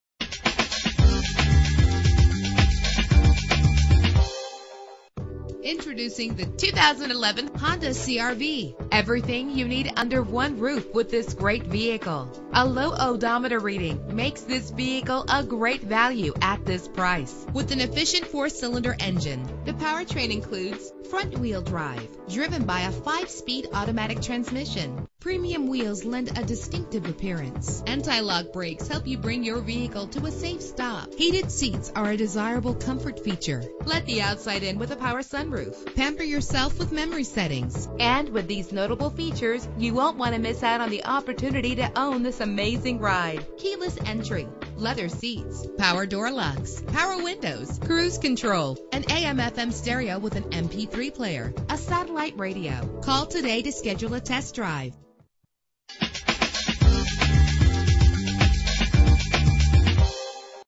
2011 Honda CR-V - Dothan AL
Year: 2011
Make: Honda
Model: CR-V
Trim: EX-L
Engine: 2.4 liter inline 4 cylinder 16 valve
Transmission: 5-Speed Automatic
Color: White
Mileage: 15262
Address:
4886 Montgomery Highway